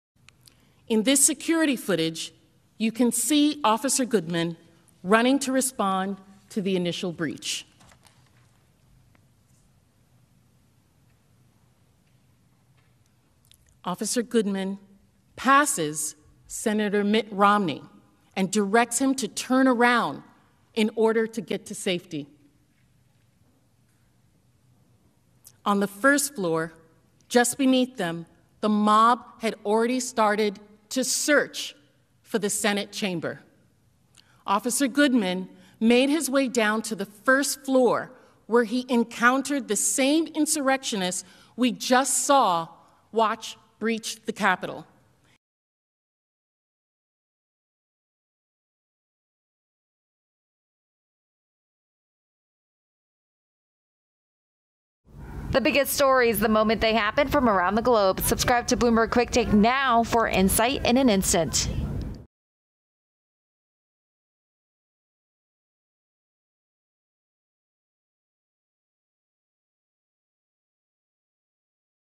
Capitol Security Footage Shows Officer Eugene Goodman Redirect Mitt Romney
House Democrats are showing video footage Donald Trump's supporters knocking down fences and fighting with police and pairing it with audio of officers making radio calls begging for backup.

Prosecutors at Trump's impeachment trial on Wednesday played police radio traffic where officers described multiple injured officers, said "they're throwing metal poles at us" and called for immediate reinforcements.

After playing increasingly desperate calls from police, Democrats showed footage of rioters breaking down windows with a riot shield to climb into the Capitol.

A never-before-seen security video from inside the building shows rioters using a wooden beam to break windows and climb into the building.

The first man climbing into the building was carrying a baseball bat and wearing body armor and is followed by a stream of people climbing through windows.